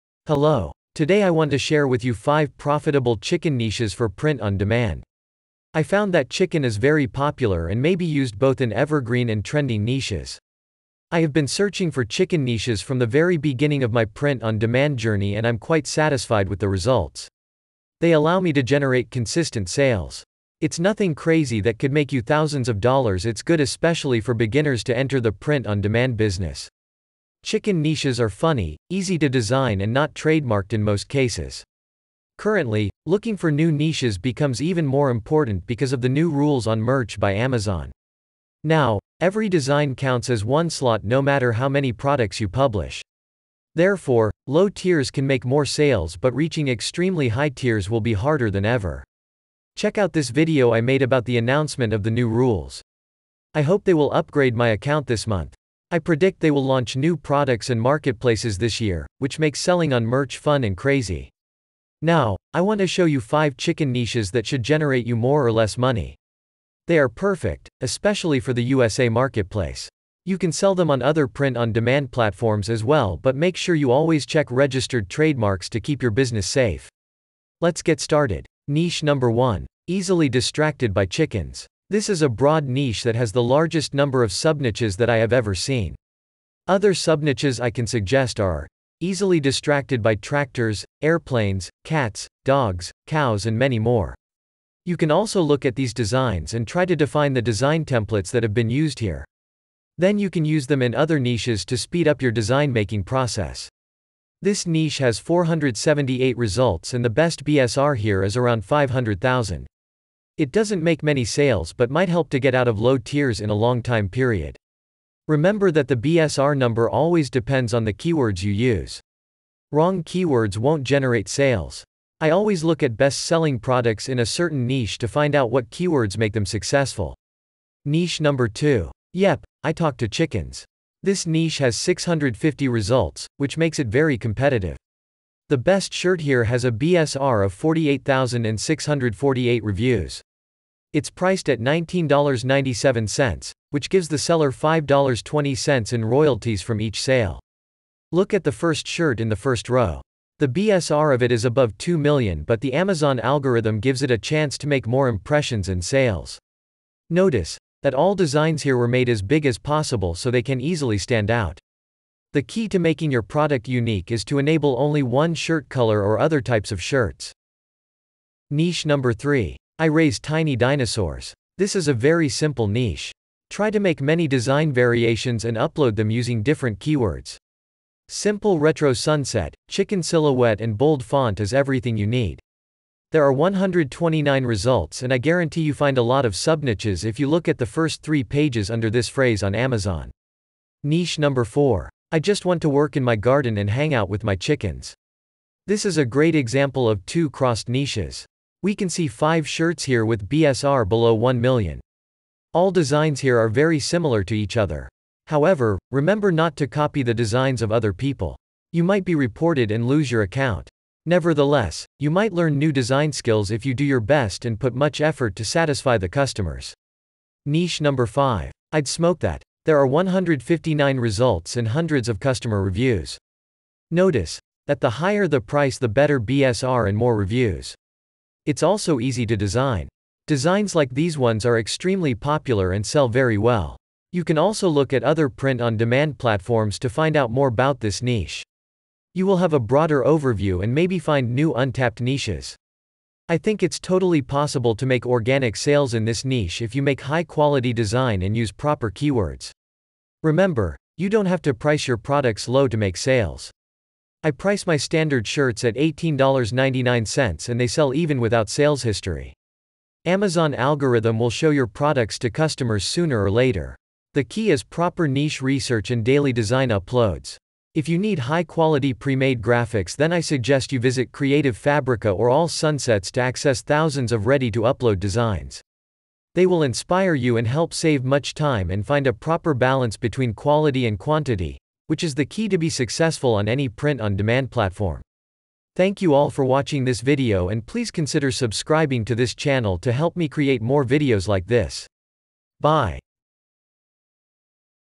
Evergreen Chicken Niches for Merch by Amazon | Print on Demand Niche Research | Make Organic Sales
In this video, I share with you 5 evergreen chicken niches for Merch by Amazon. Making proper niche research for print on demand is the key to be successful. I found some low competition niches that are good even for beginners and can generate organic sales. Check it out!